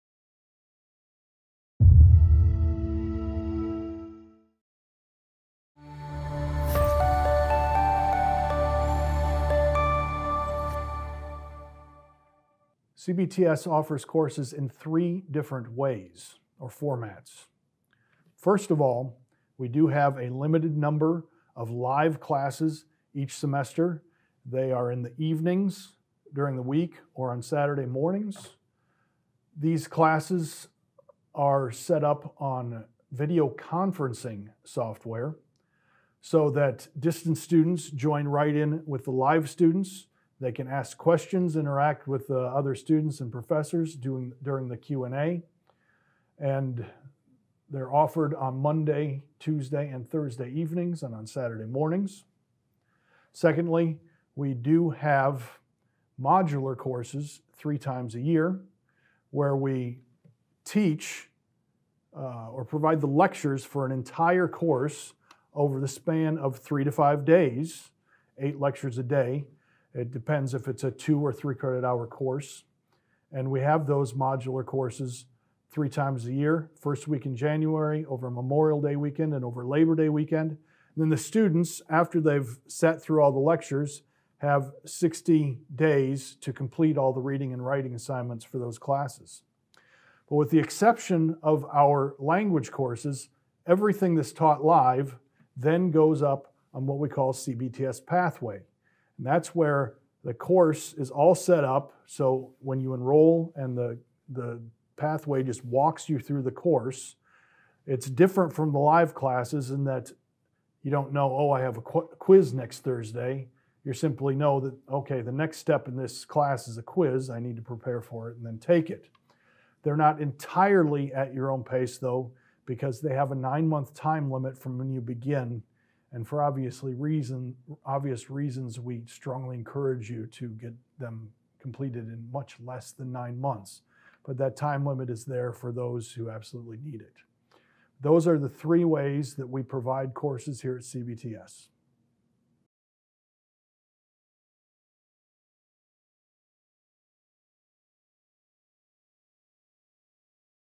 3 Ways to Take Classes
Learn about the 3 ways you can take classes at Covenant Baptist Theological Seminary.